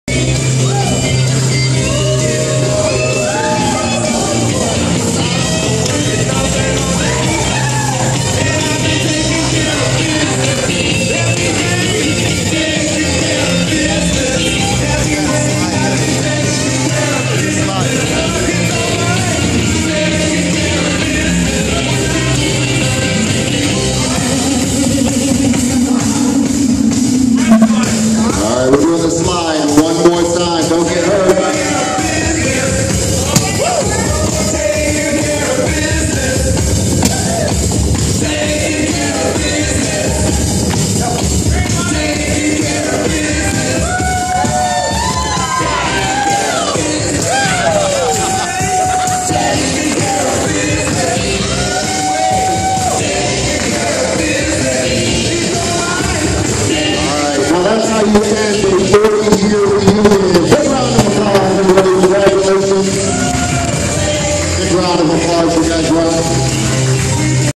Goodtimes Ent
Man jump rope and human slip n slide